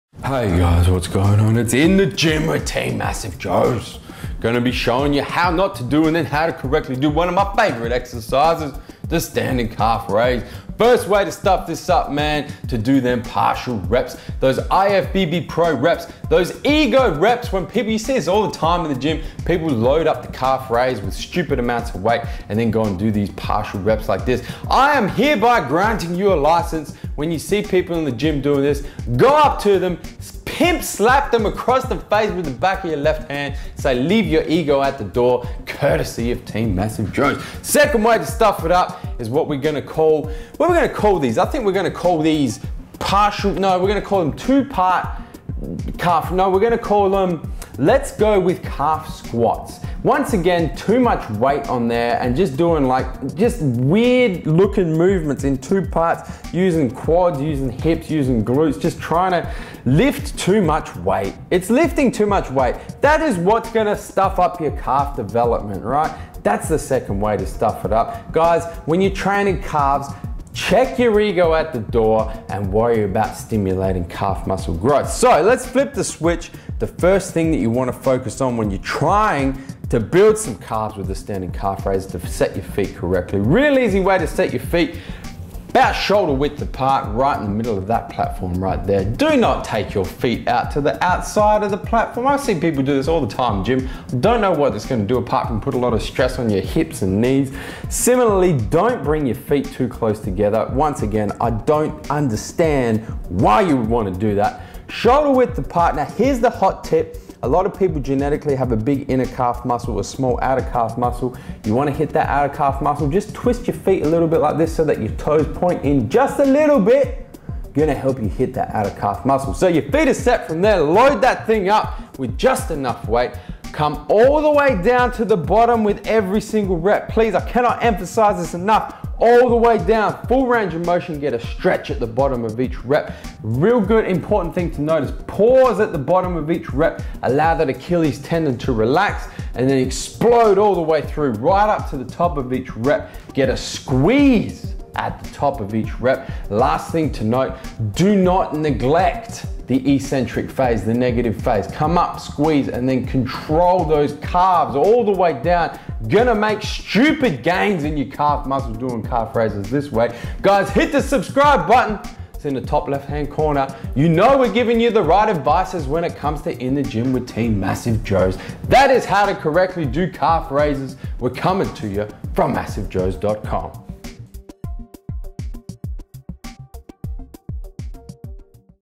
Standing Calf Raise Instructional Video - In The Gym With Team MassiveJoes - MassiveJoes.com How to
MassiveJoes.com Owner, Joseph Mencel, shows you how to, and how not to, correctly do standing calf raises to help you build MASSIVE calves... ah, I mean COWS!